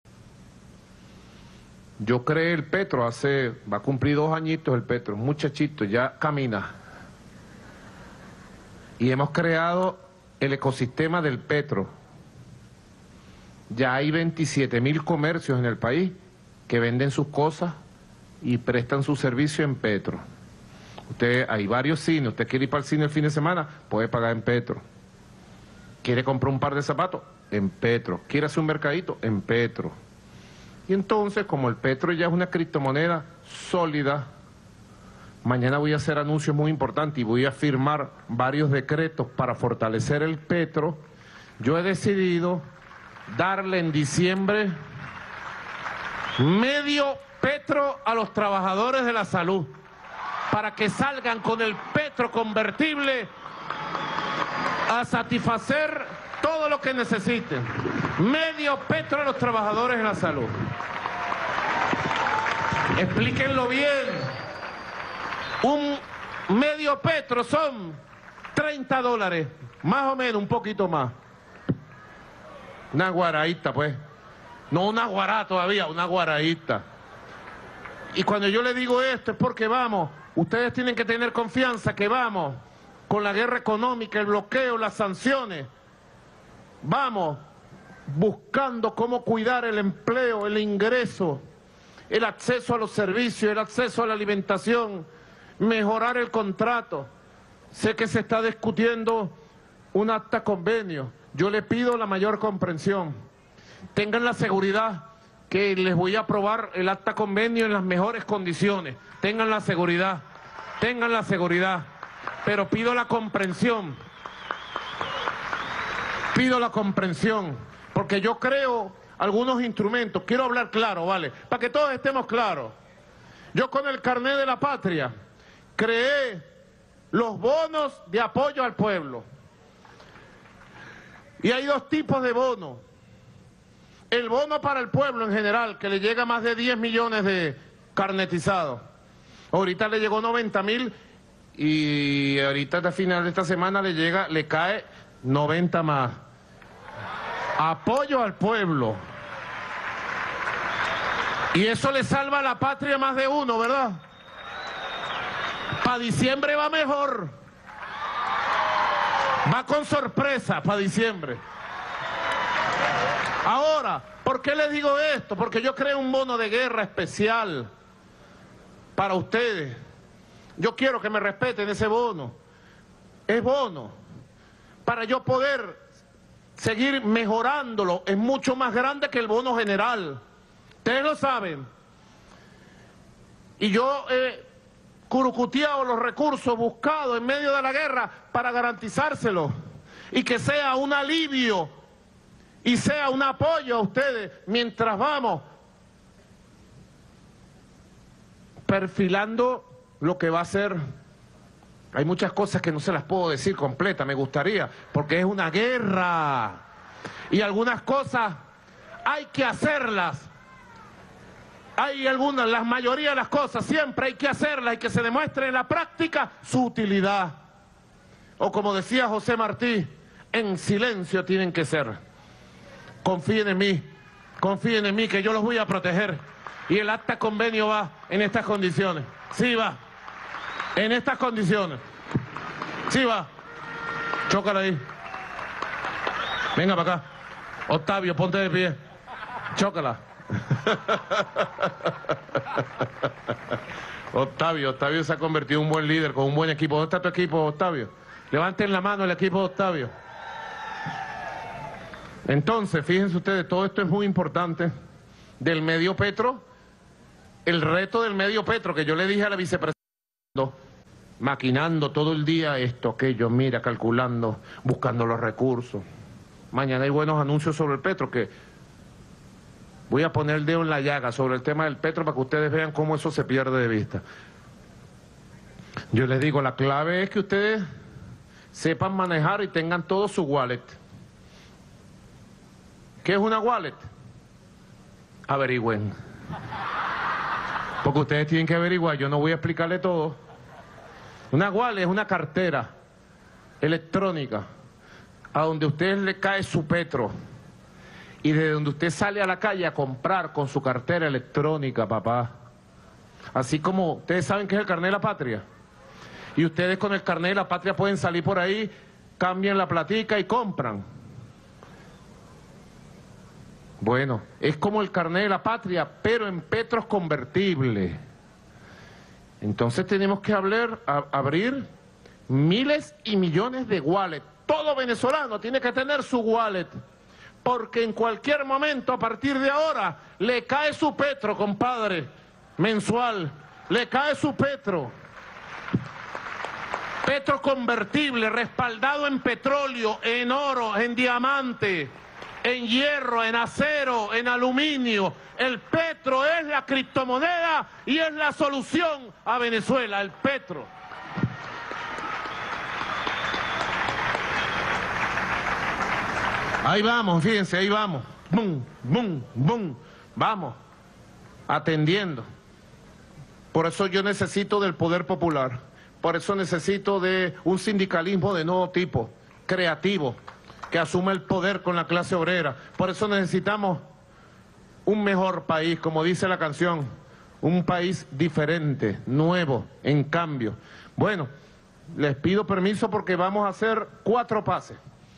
Maduro confirma que dará medio Petro a personal de salud y anima a todos a abrir su wallet
El Presidente venezolano, Nicolás Maduro, informa este 19 de noviembre de 2019 en una reunión con médicos y personal de salud en el Palacio de Miraflores, que el día 20 hará anuncios sobre el tema del Petro, y que dicho personal recibirá medio petro como bono decembrino. Confirmó que medio Petro es equivalente a 30 dólares e insto a todos los venezolanos a investigar qué es un wallet y abrir su wallet pues en cualquier momento podía llegarles bonos en petros. También informó de un nuevo bono a través del Sistema Patria de Bs. 90 mil en los próximos días, y de nuevos bonos para el mes de diciembre.